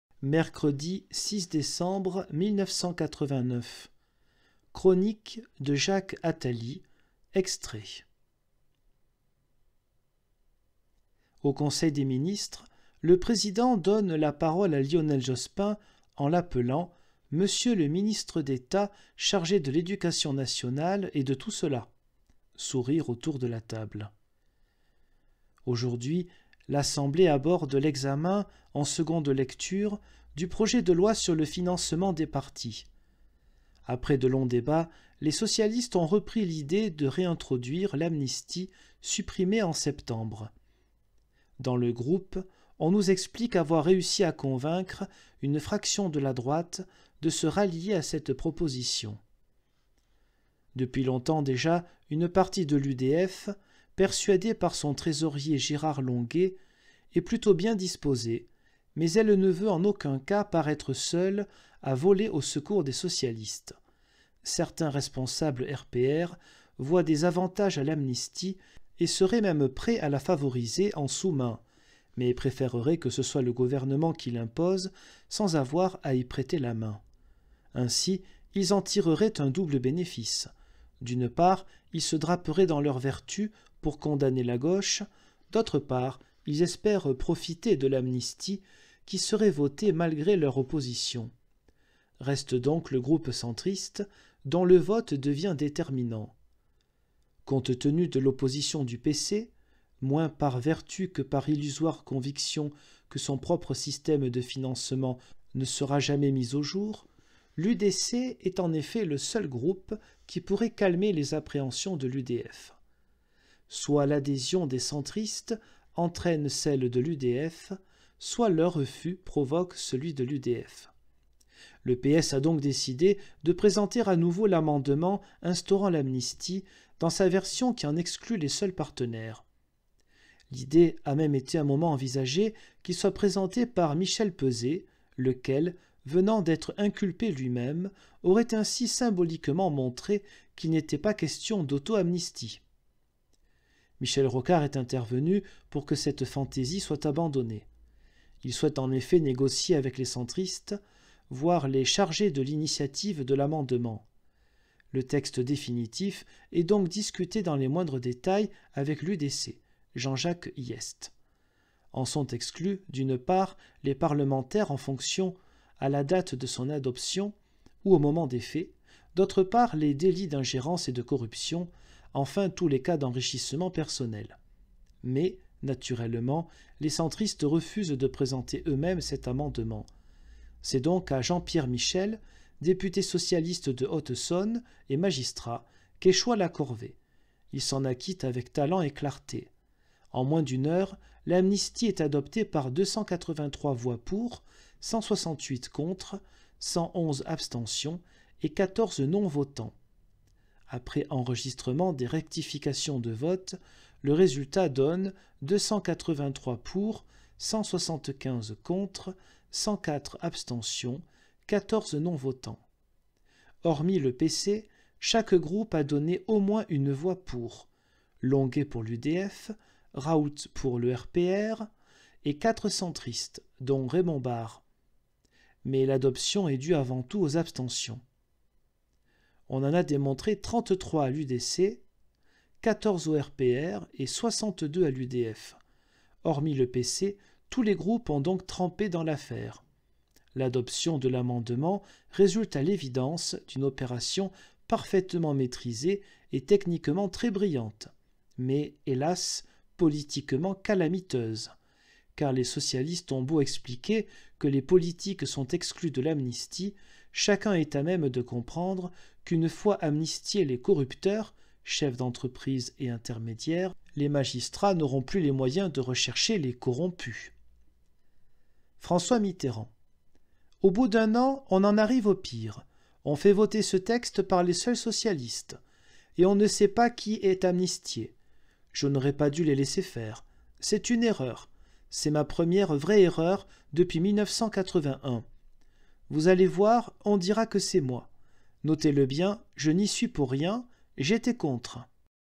1989. 06 décembre. Chronique de Jacques Attali
Extrait de :

Jacques Attali, "Verbatim III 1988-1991". Fayard, 1995, p. 358-359